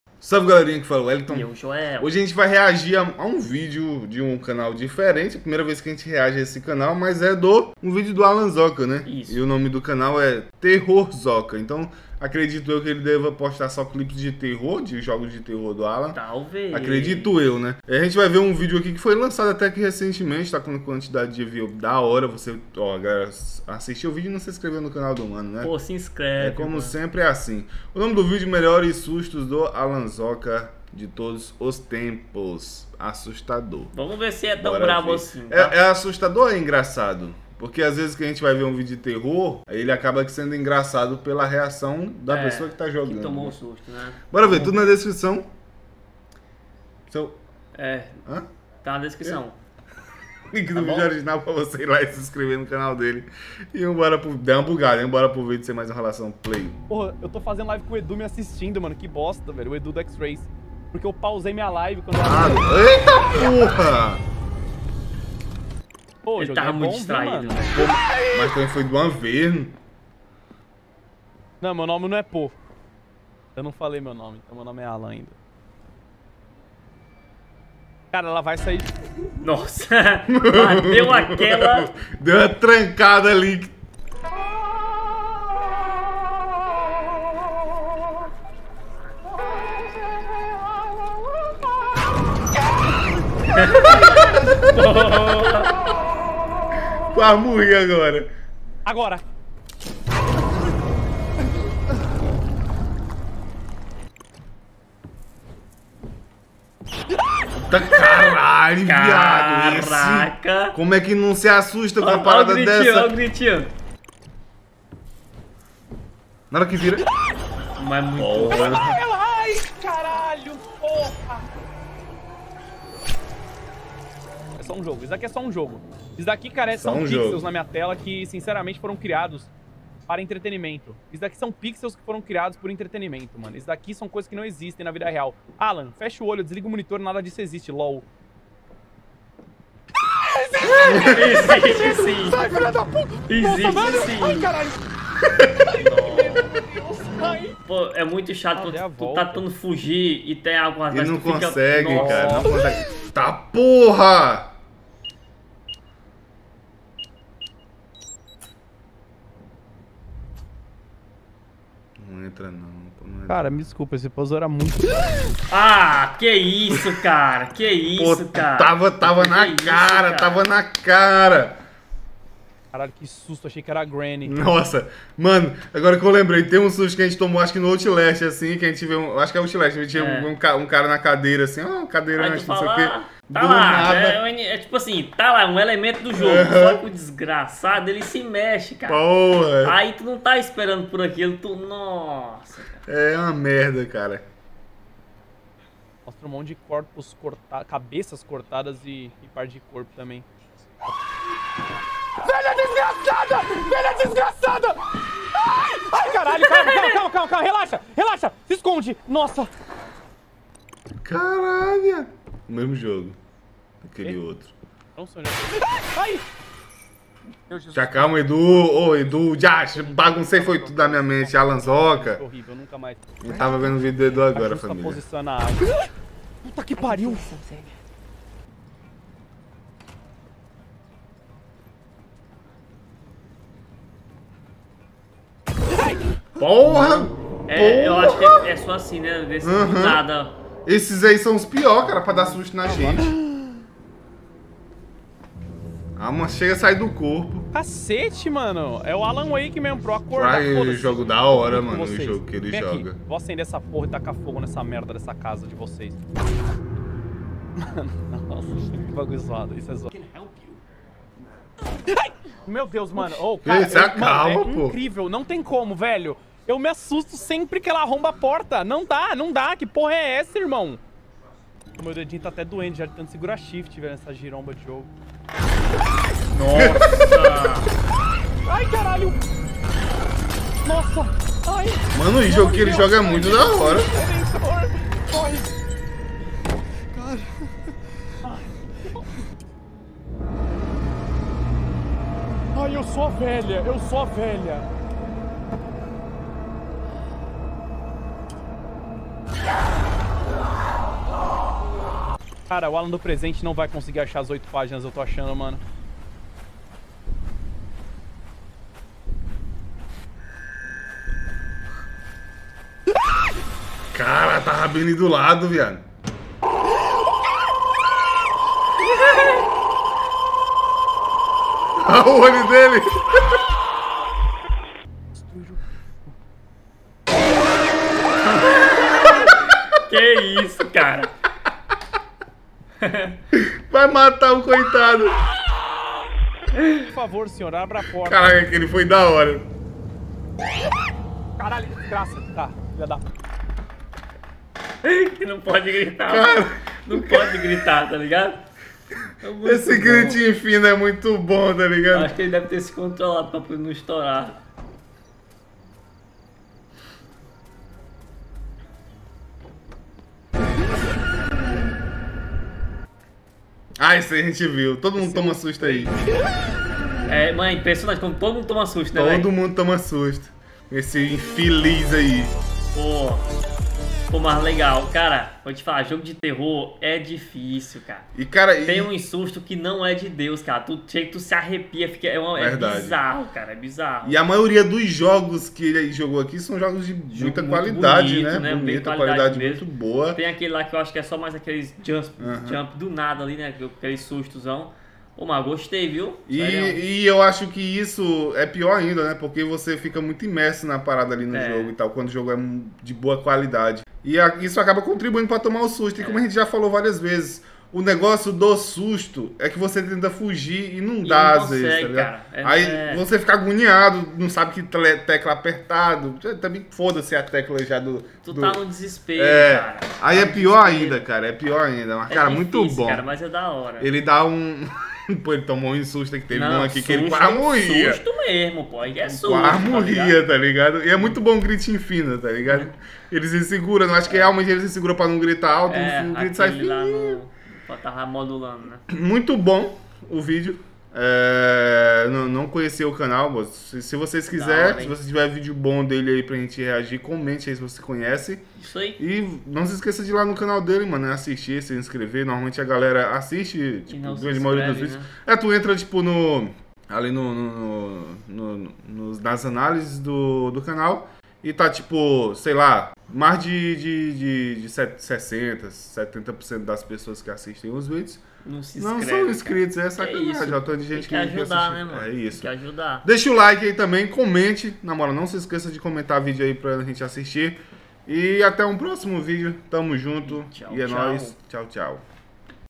REACT MELHORES SUSTOS do ALANZOKA DE TODOS OS TEMPOS (ASSUSTADOR)
Hoje reagimos a um vídeo lá do canal - Terrorzoka

Canal de lives na Roxinha: periodico_lives

Você nos motiva a continuar :)